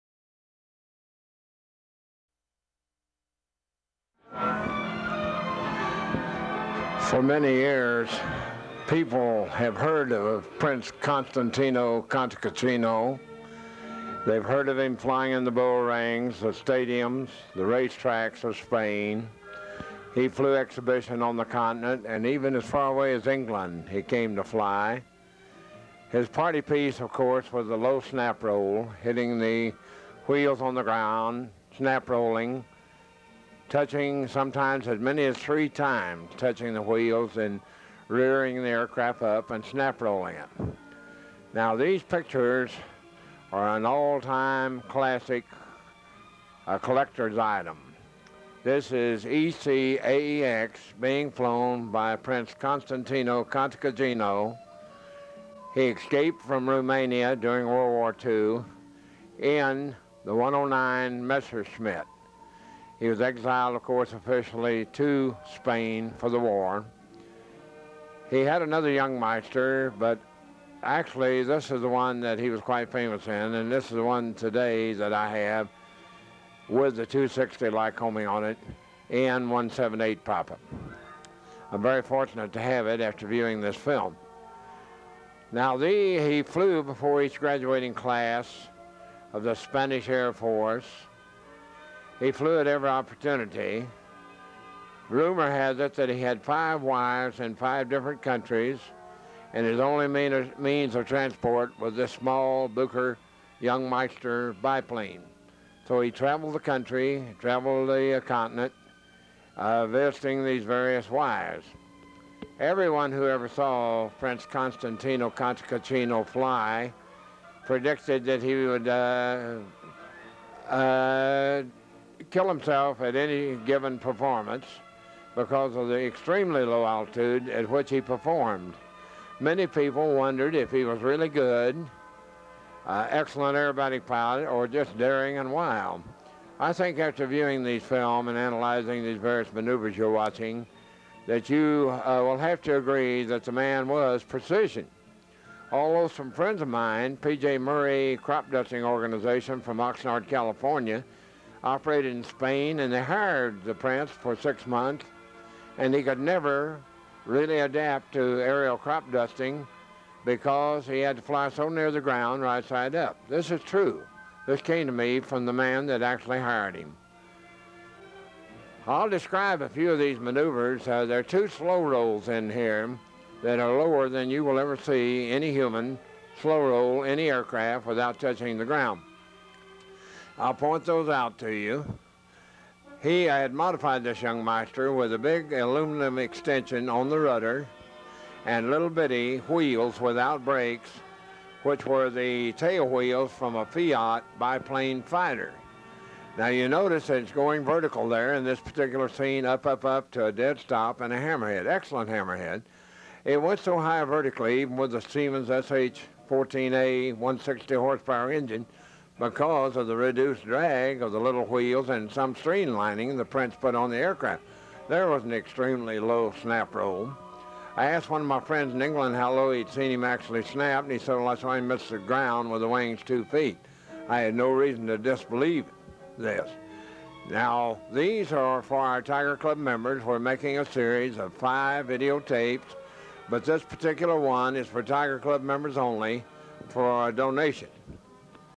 Constantin Cantacuzino Part 1 of 2
Narrated by Frank Price.
Constantin Cantacuzino (nicknamed Bâzu; November 11, 1905--May 26, 1958) was a Romanian aviator, one of his country's leading World War II fighter aces, and a member of the Cantacuzino family.